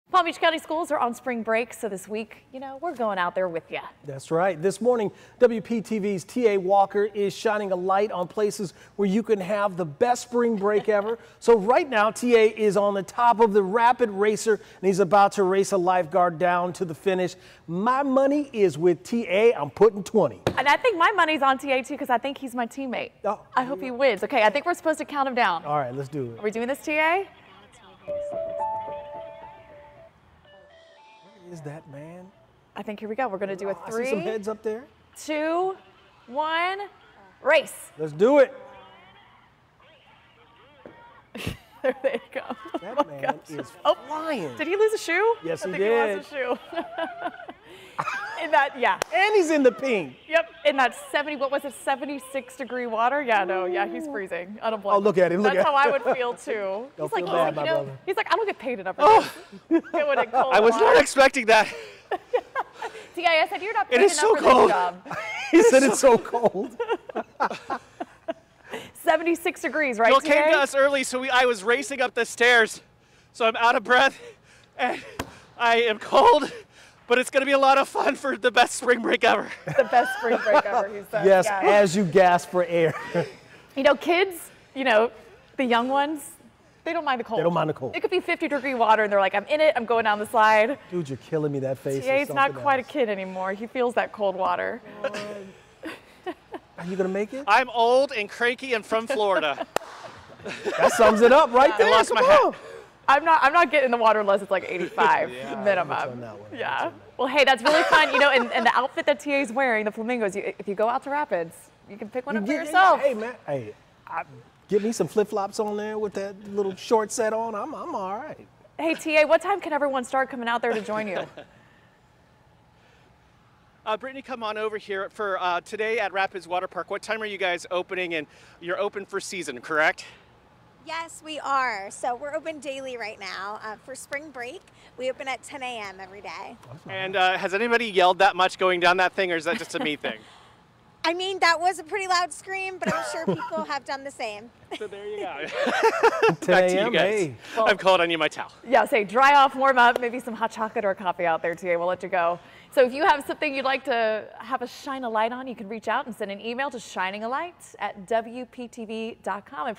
Racing down the Rapids Racer
Palm Beach County schools are on Spring Break so this week we are going with you.  This morning WPTV’s T.A. Walker is Shining A Light on places where you can have the “Best Spring Break Ever”.  So right now, T.A. is on top of the Rapid Racer and he is about to race a lifeguard down to the finish line